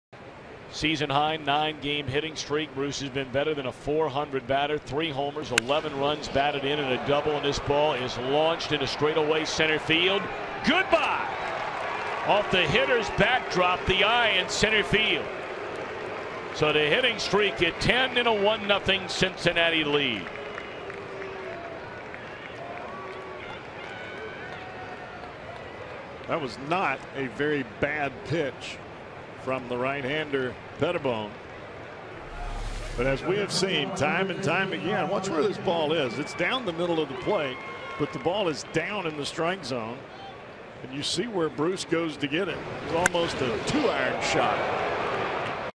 2013/05/19 Bruce's solo blast
CIN@PHI: Bruce's solo shot gives Reds the early lead